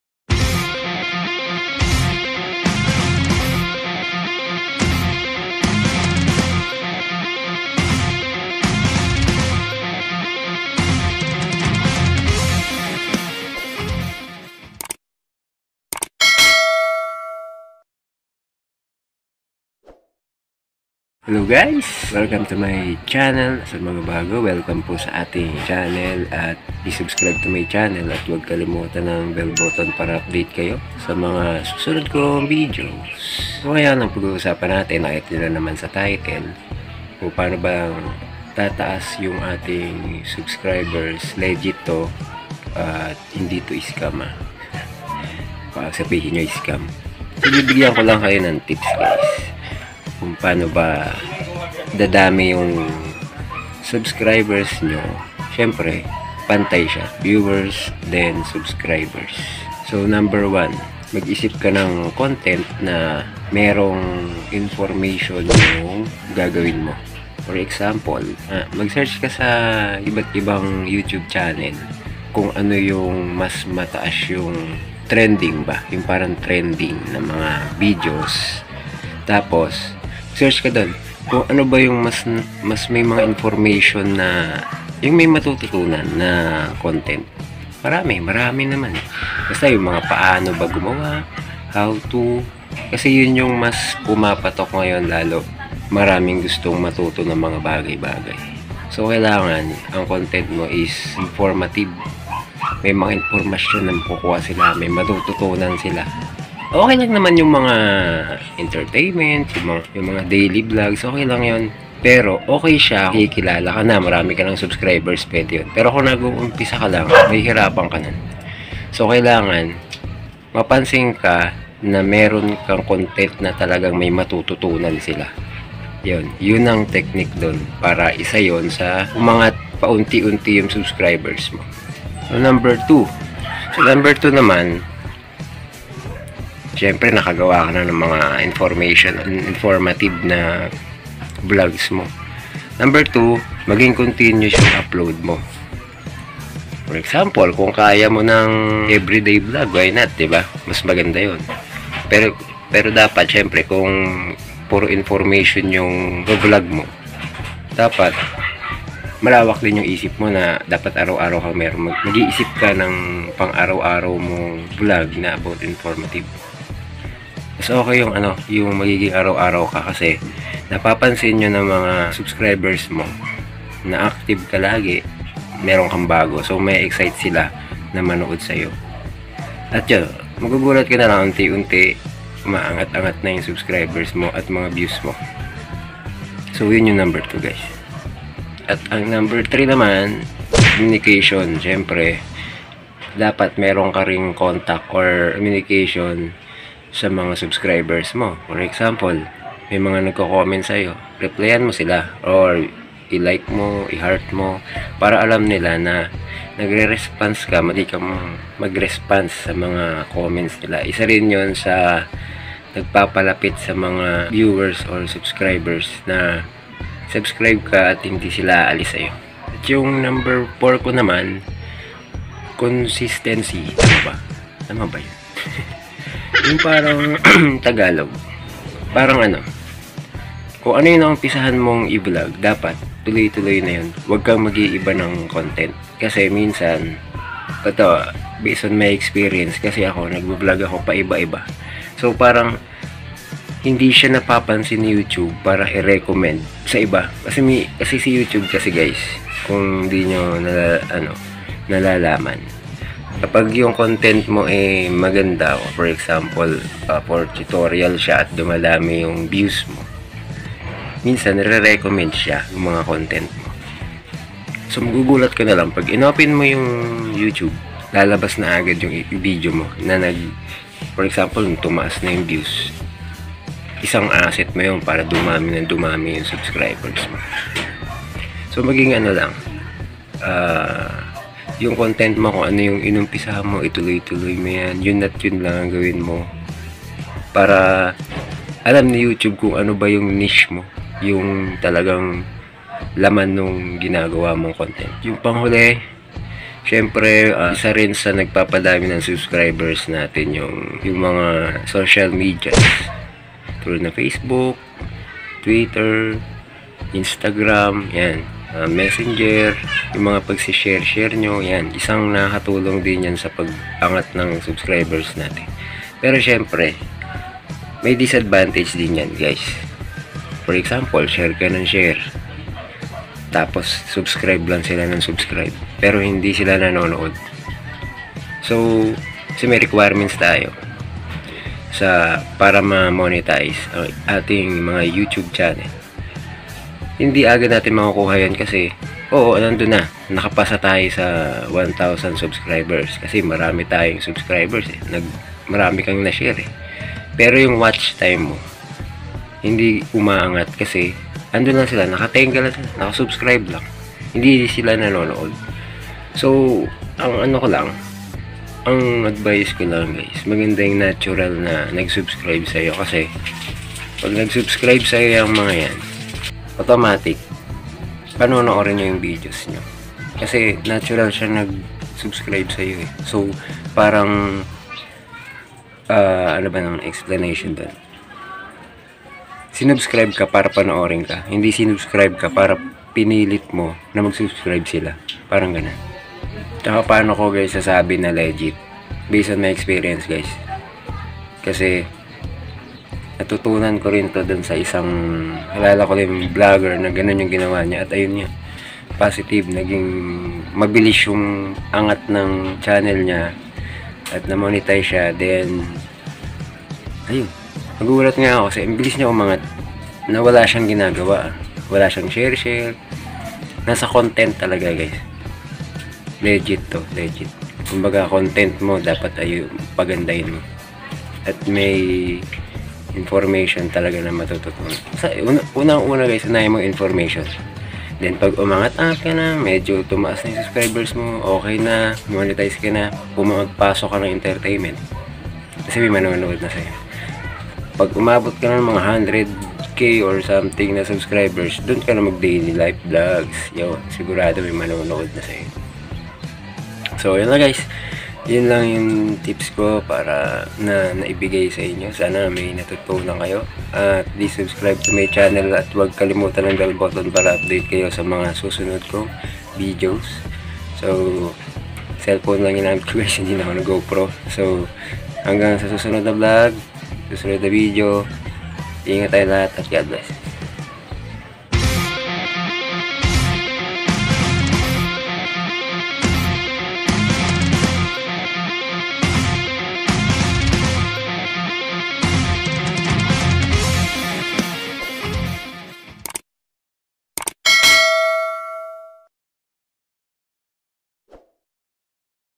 Paano Umangat o Tumaas ang Subscribers at Views mo naturally Legit!!! No Scam!!!
Hello mga ka tol, gusto ko lang ishare sa inyo ung mga natutunan ko kung paano legit na umangat at dumami ang views ng ating mga channel. panoorin niyo hanggang dulo para makakuha rin kayo ng idea.. maraming salamat mga ka Tol.